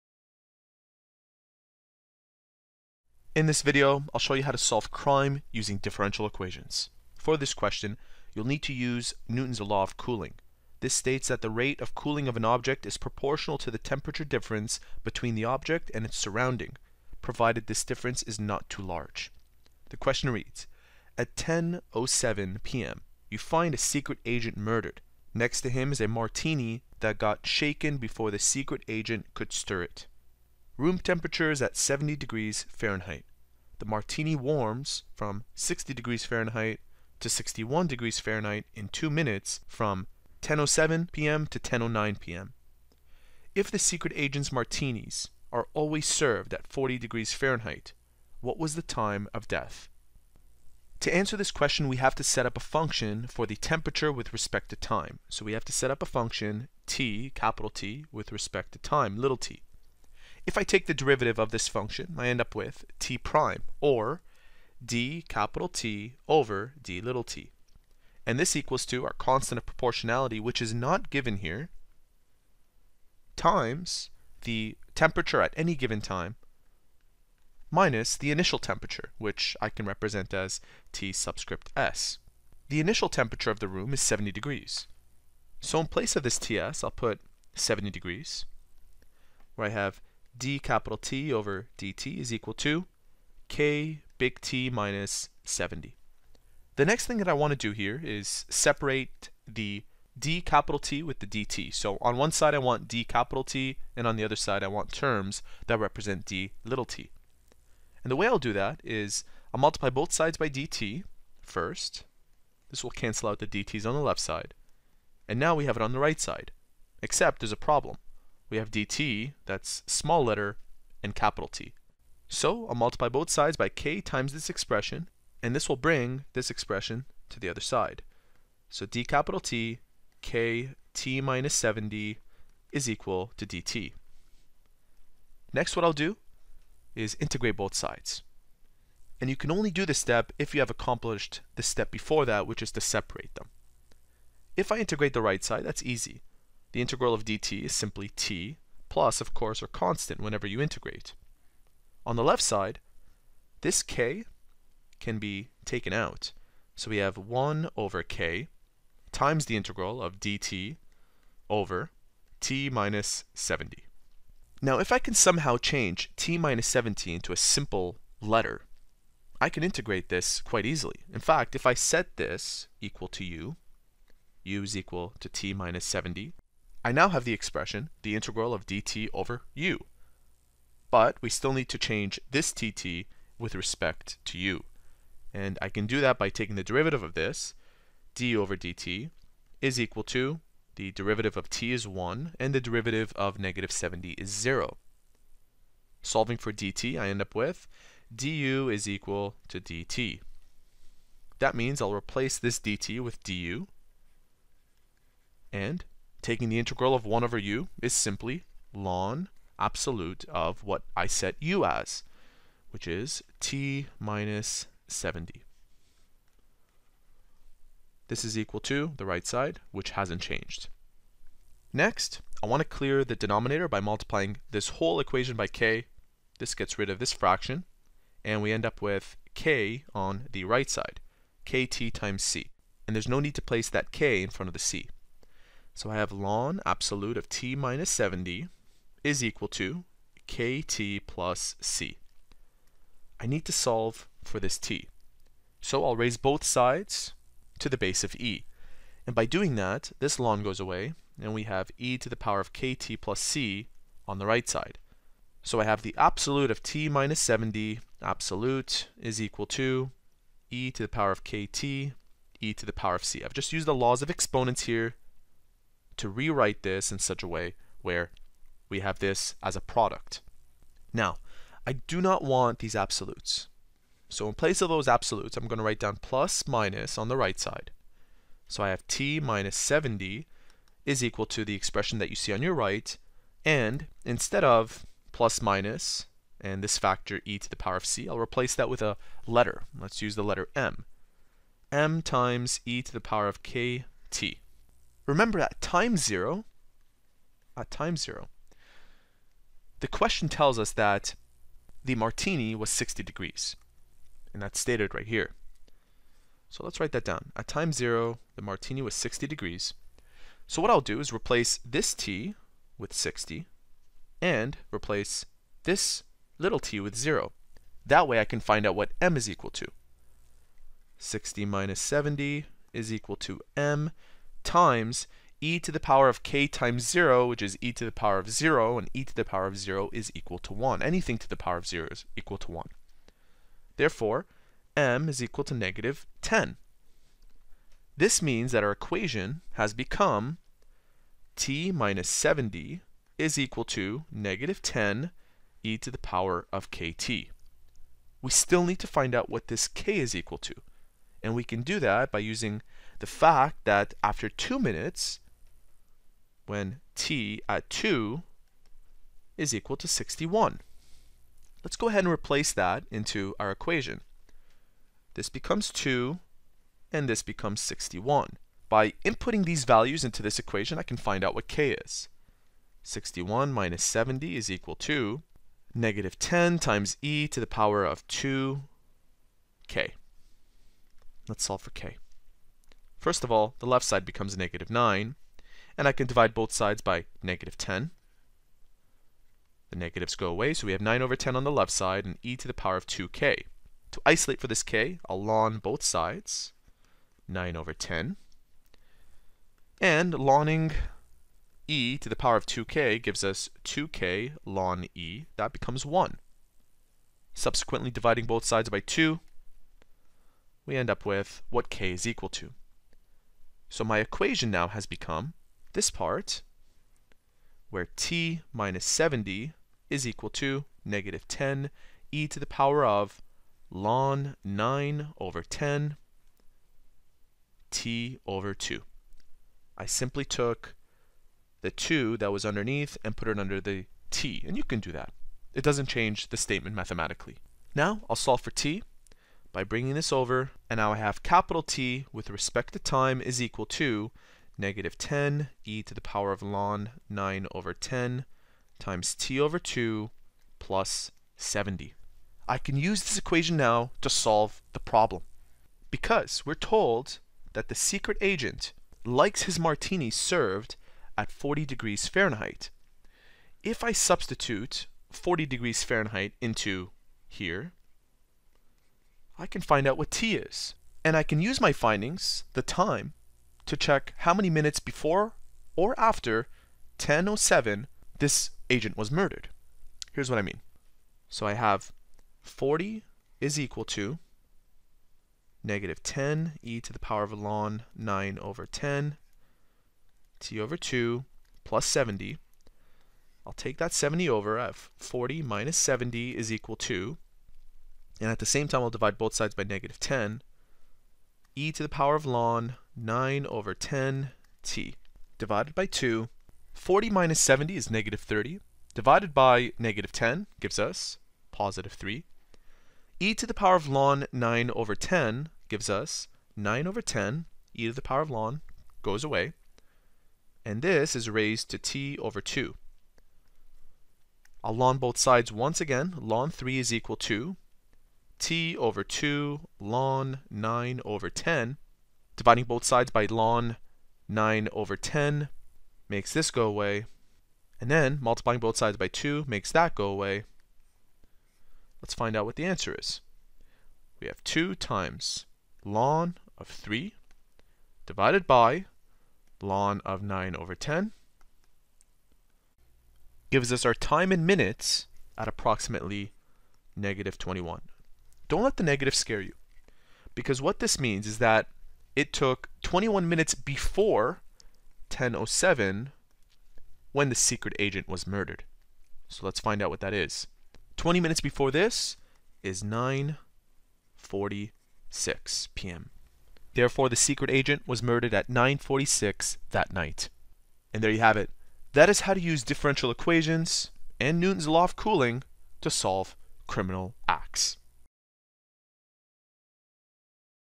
📚 How to solve a crime using differential equations
🤔 Still stuck in math? to start asking questions.

For this question, we'll need to use Newton’s Law of Cooling. This law states that the rate of cooling of an object is proportional to the temperature difference between the object and its surroundings (provided this difference is not too large).

Q.   At 10:07 pm, you find a secret agent murdered. Next to him is a martini that got shaken before the secret agent could stir it. Room temperature is 70 °F. The martini warms from 60 °F to 61 °F in the 2 minutes from 10:07 pm to 10:09 pm. If the secret agent’s martinis are always served at 40 °F, what was the time of death?